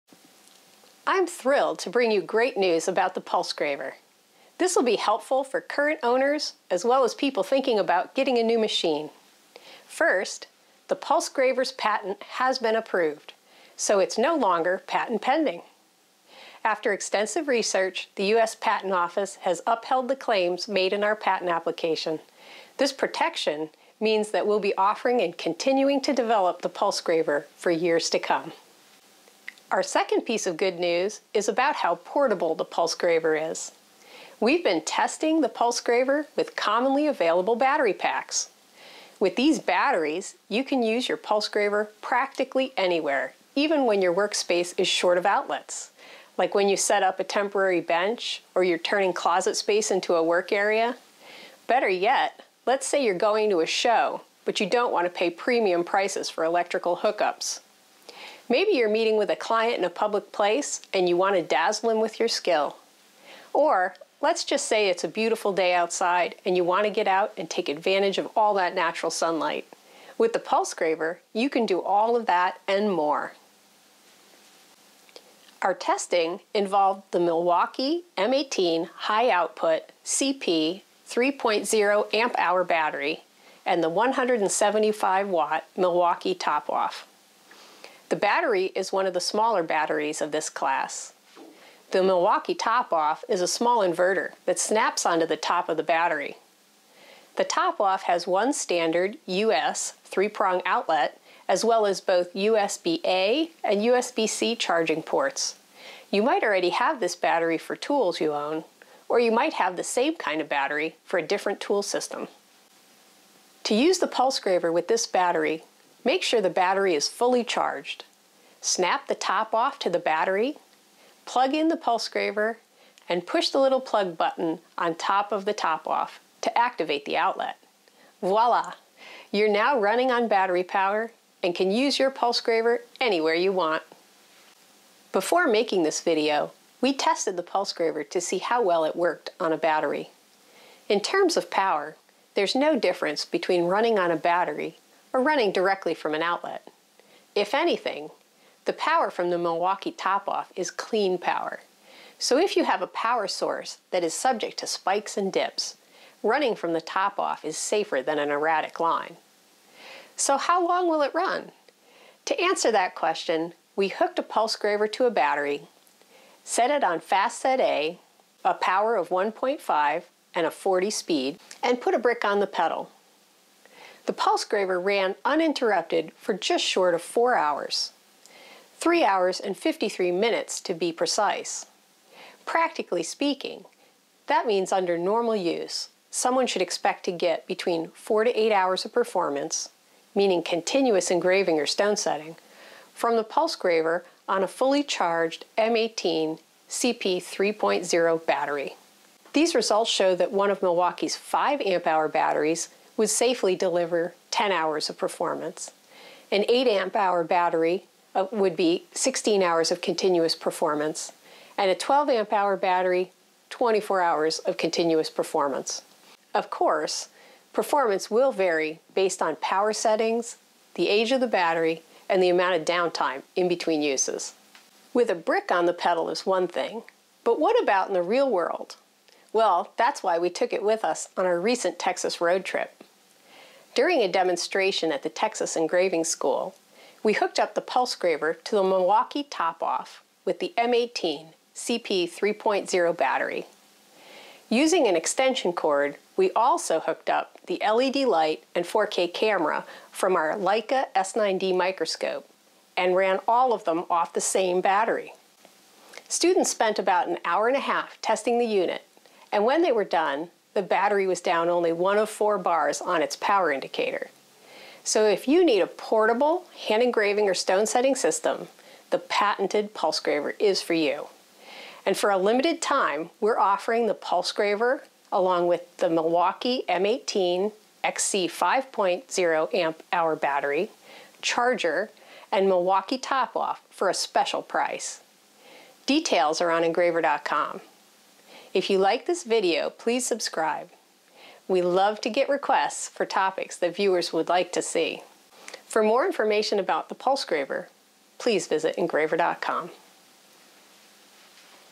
Patented Portable PulseGraver Runs on a Milwaukee Battery
Tira Mitchell announces that the PulseGraver's patent was formally approved.

Tira also explains that the all-electric PulseGraver® airless engraving and stone setting system can be run on a battery.  Precision Artistry LLC tested the PulseGraver® on a Milwaukee M19 CP3.0 battery and Milwaukee's Top-Off 175W inverter.  In-house testing exceeded expectations, and a live trial during the recent Texas Road Trip to Wes Griffin's Texas Engraving School stood up to real-world use.

There are a limited number of PulseGravers with Milwaukee XC5.0 battery kit combinations on sale  This special promotion only ships to the continental United States due to shipping limitations with lithium batteries and differences with charger types in non-US countries.